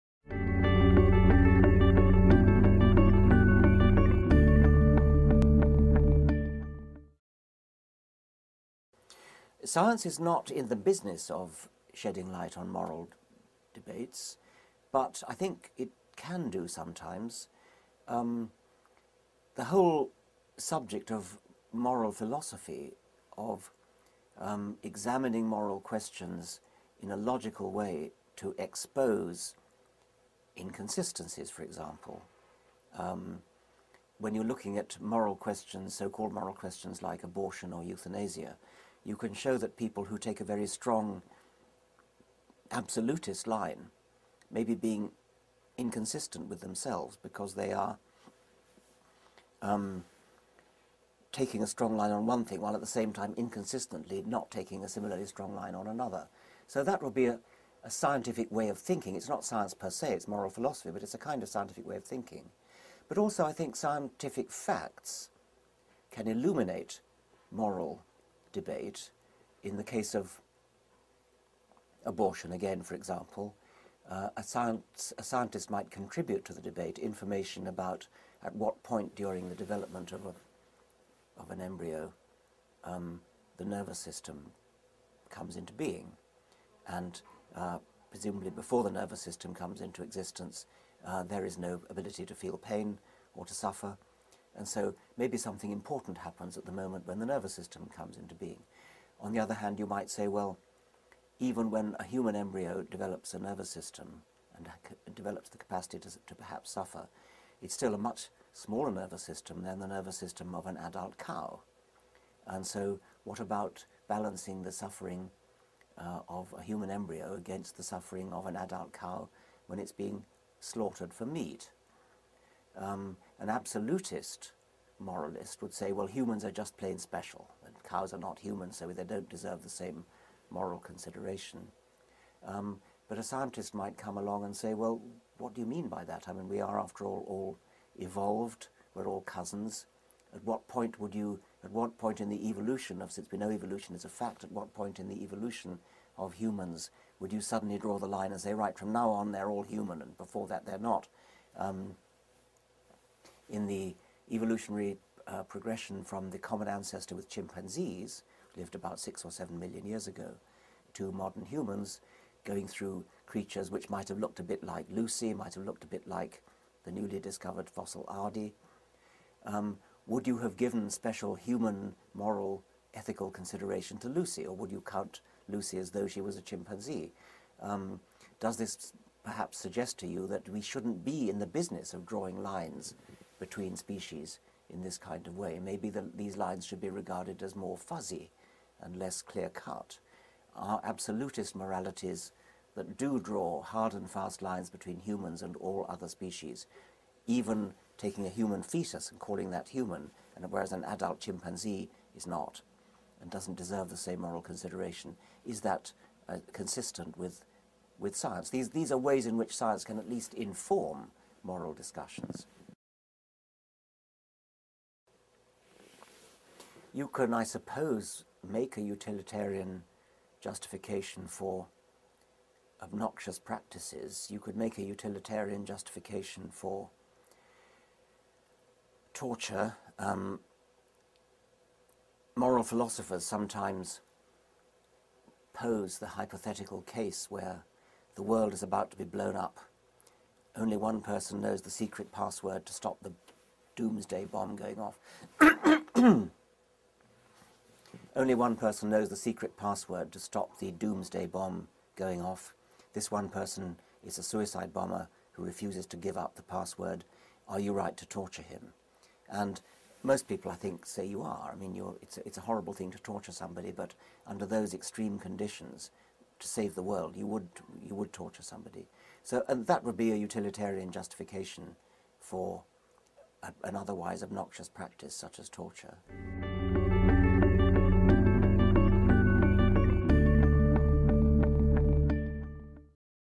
Richard Dawkins - Deixando a ciencia informar a moralidade
Enquanto a ciência está indelevelmente distinta do campo da ética, Richard Dawkins acredita que há uma série de maneiras em que poderia beneficiar muito a nossa capacidade de compreender e reparar o sofrimento no mundo.